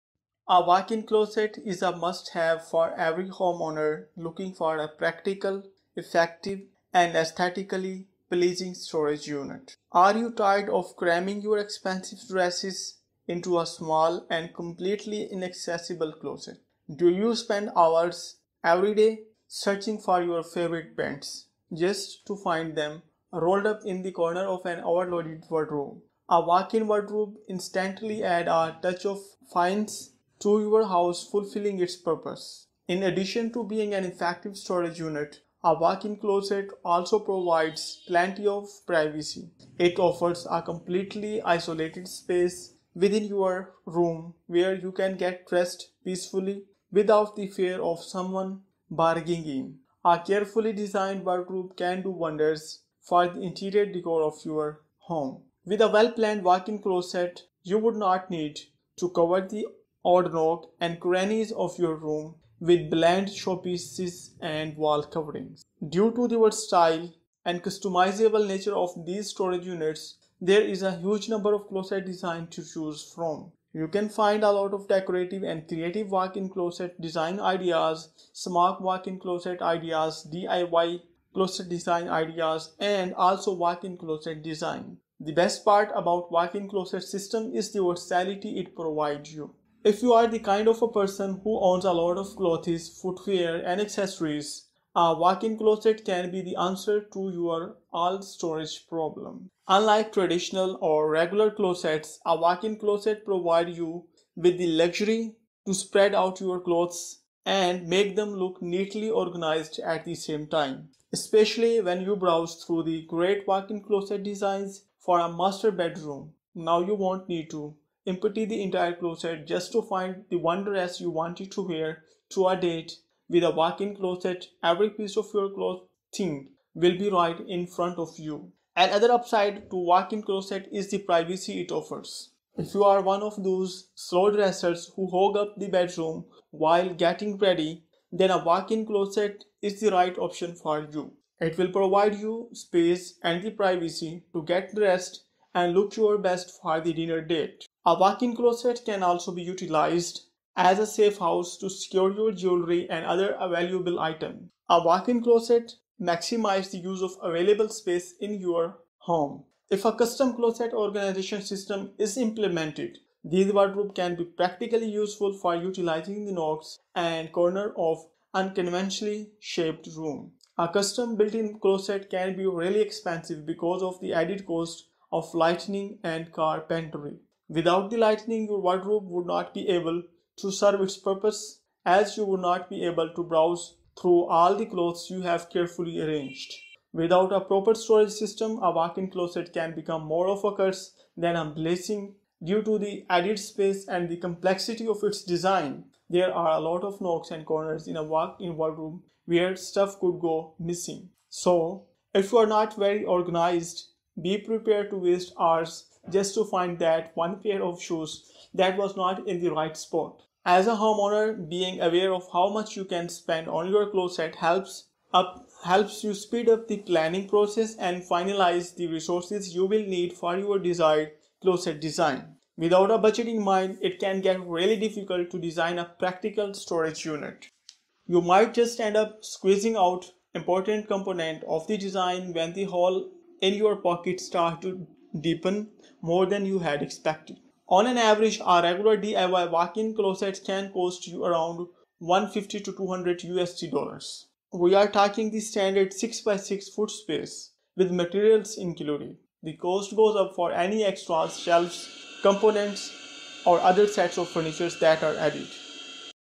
Walk in Closet Design Ideas- Modern Closet Designs- Walk-in Wardrobe Ideas
Walk in Closet Design Ideas- Modern Closet Designs- Walk-in Wardrobe Ideas: Closet Design Ideas- Wardrobe Designs
Hello, friends hope you all are fine. Today I bring you walk-in closet design ideas. I sure these closet designs going to give you great information and inspiration.